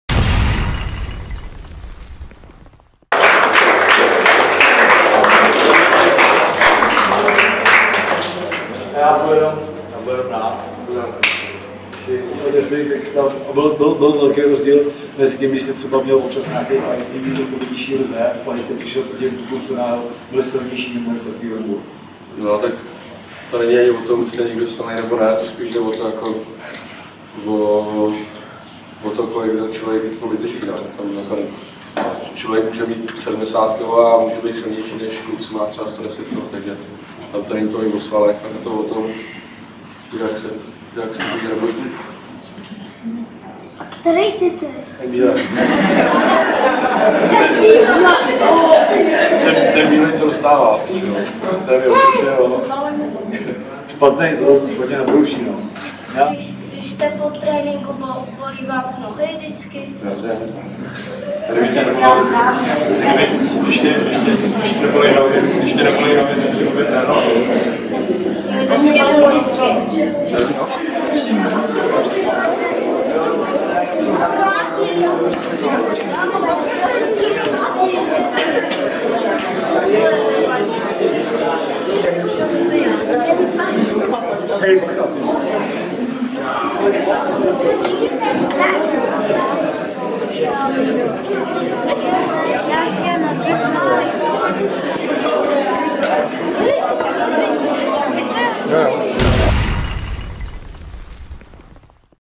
Radek Smoleňák navštívil Hokejovou školu
Odstartoval již třetí ročník Hokejové školy Rytířů Kladno a hned v úvodu potěšil svou návštěvou hokejové talenty útočník Radek Smoleňák.  Forvard, který v české reprezentaci oblékl i dres s kapitánským "C",, rozdal mnoho cenných rad a podpisů.